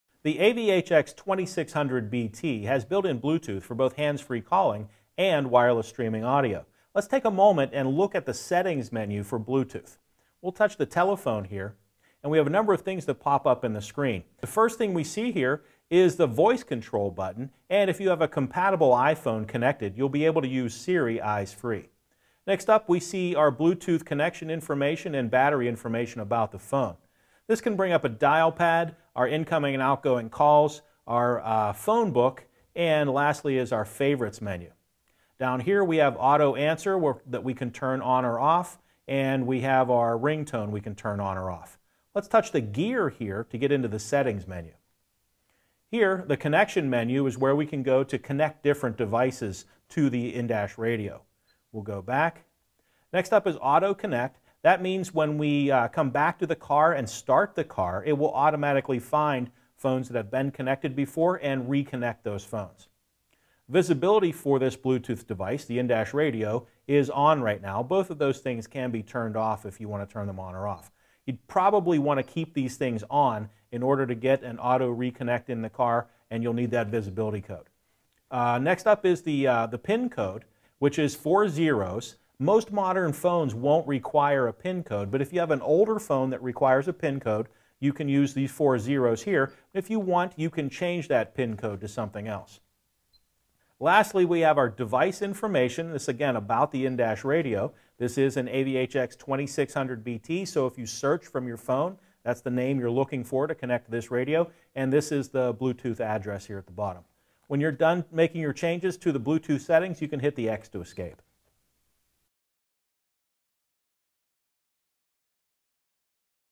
How To - AVH-X2600BT - Change Bluetooth Settings
Learn how to change the Bluetooth Settings on the Pioneer AVH-X2600BT.